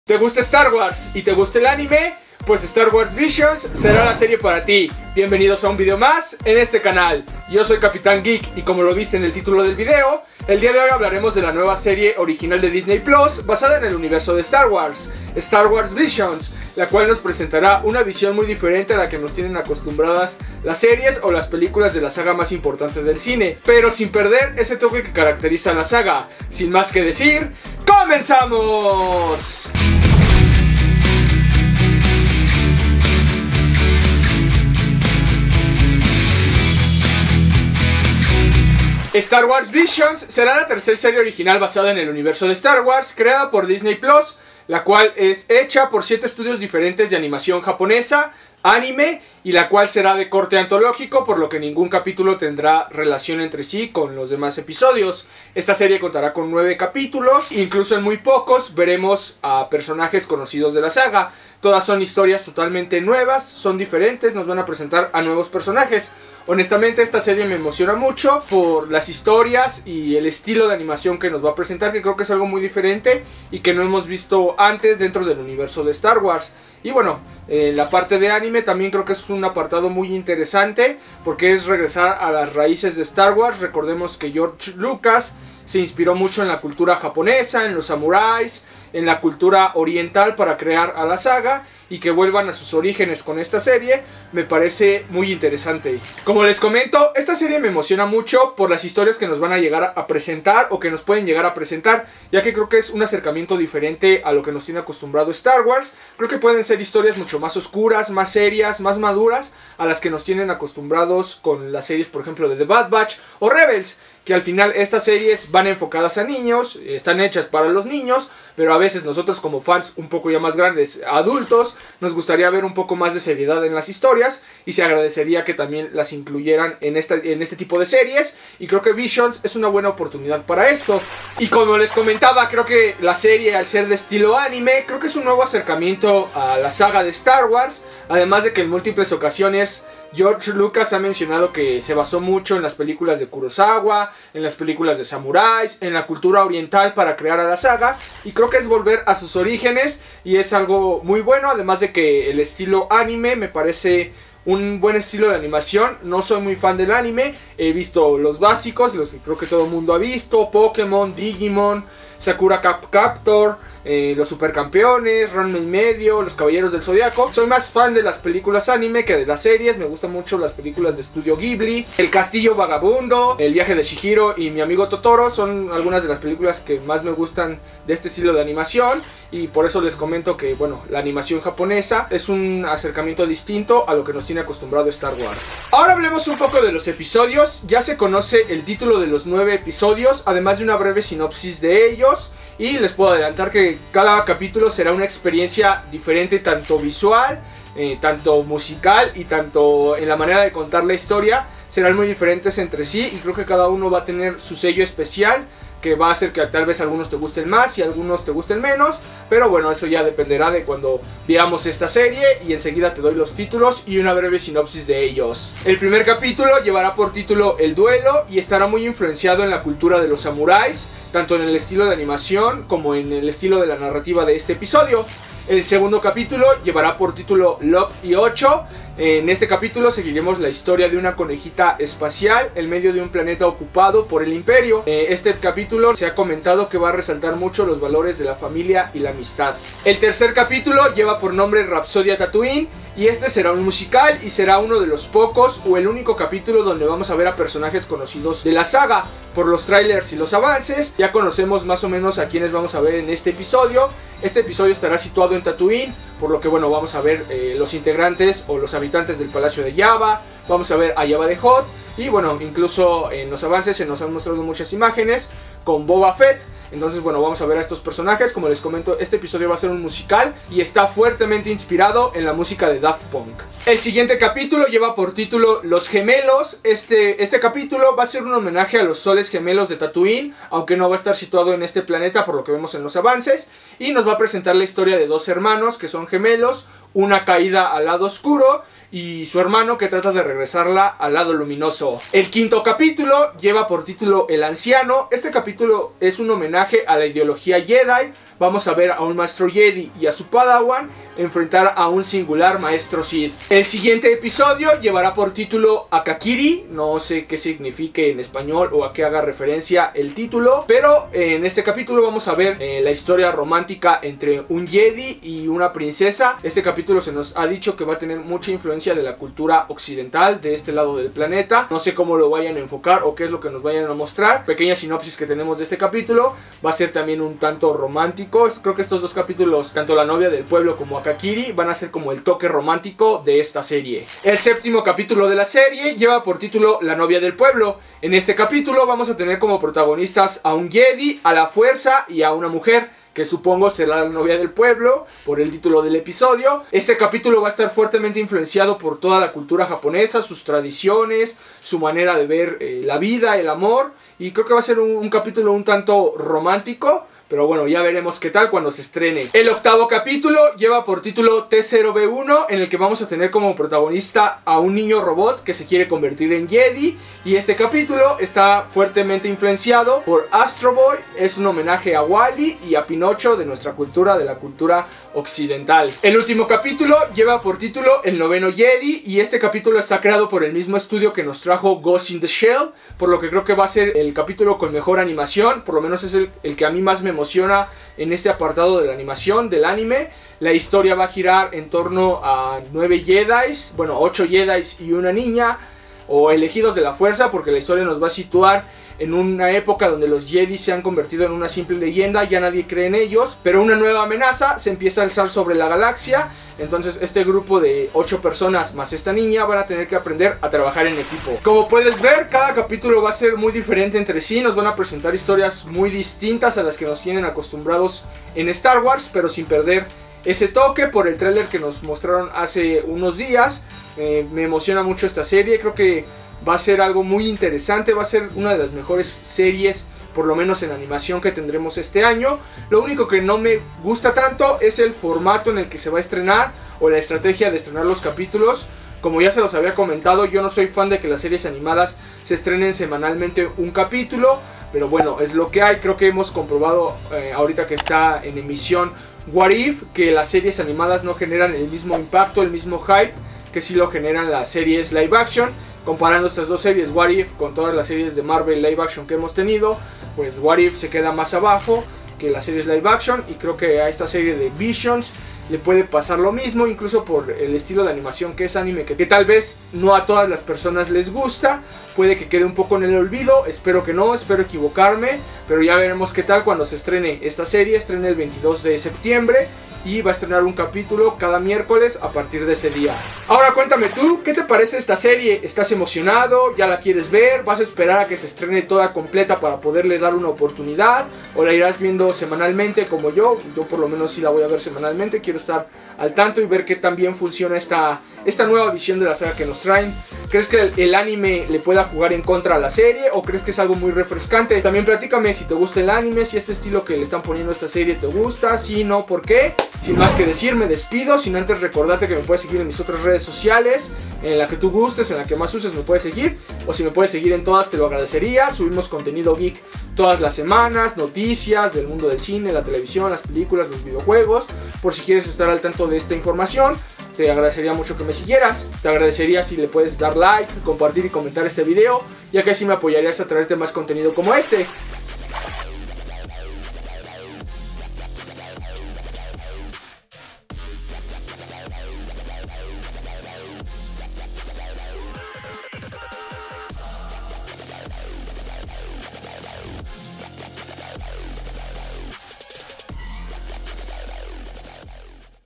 UNA SERIE MUY DIFERENTE I ¿QUÉ ES STAR WARS VISIONS? 🤔💥🚀StarWars
La tercera serie original basada en el universo de StarWars creada por DisneyPlus llega en septiembre con una Visions muy difieren a lo que la saga nos tiene acostumbrados, pero sin perder ese toque que la caracteriza. Acompáñame averiguar que es Visions la serie que regresa a las raíces de la saga mas importante del cine.

Así me apoyas a traer mas contenido como este ¡Muchas Gracias! 😁